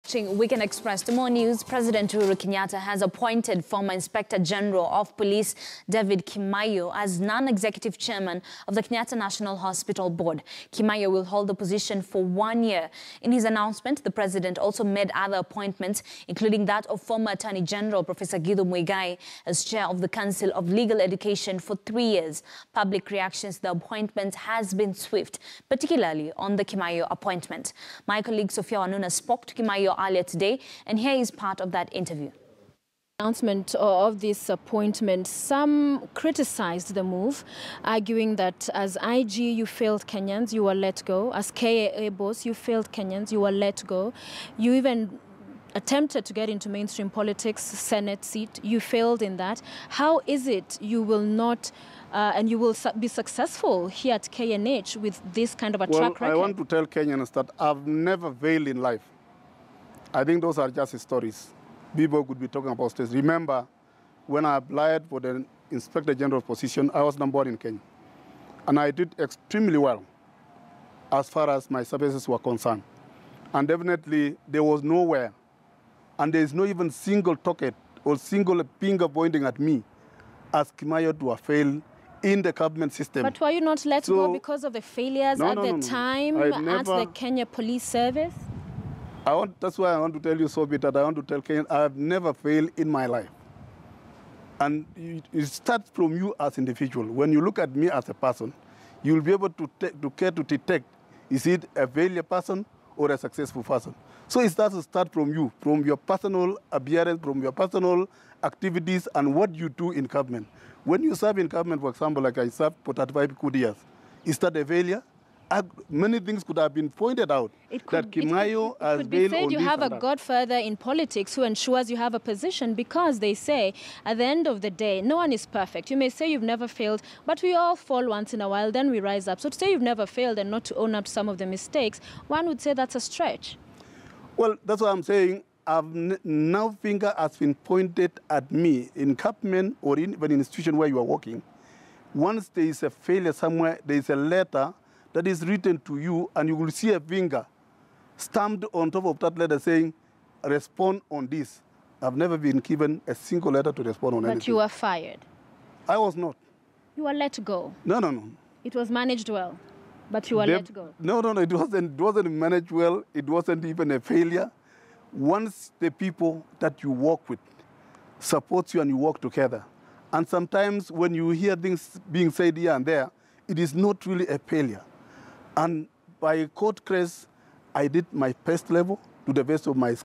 President Kenyatta appoints former IG David Kimaiyo as non-executive chair of KNH
President Uhuru Kenyatta has appointed former inspector general of police David Kimaiyo as non-executive chairman of the Kenyatta national hospital board. Kimaiyo will hold the position for one year. In his announcement, the president also made other appointments including that of former attorney general prof. Githu Muigai as chair of the council of legal education for three years. Public reaction to the appointments has been swift, particularly on the Kimaiyo appointment.